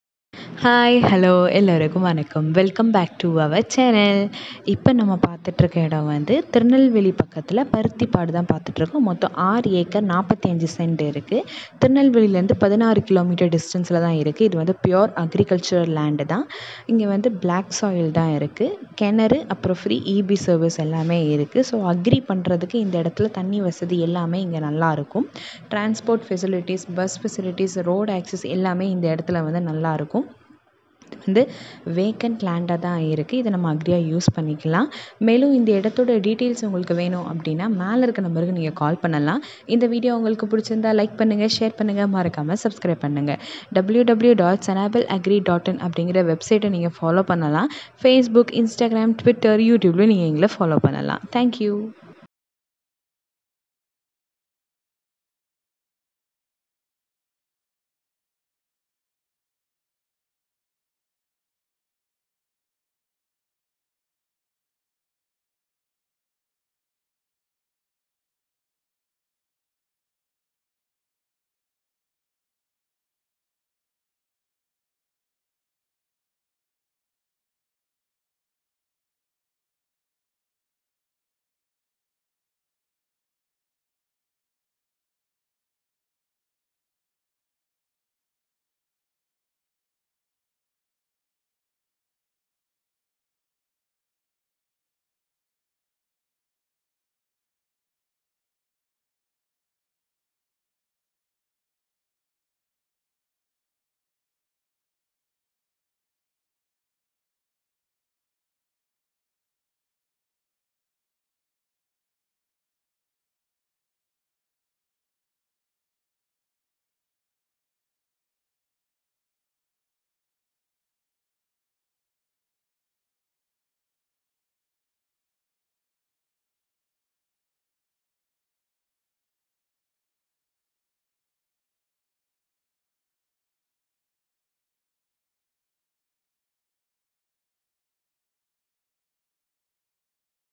NO: 100 * Agricultural land for sale * Tirunelveli ( 6. 45 acres)
http:\\sanaabilagri.in\
Agricultural land for sale
Tirunelveli
location : parthipadu
6.45 acres
tirunelveli 16km
blacksoil
well and free services
good water sources
good road acess
vacant land
pure agri land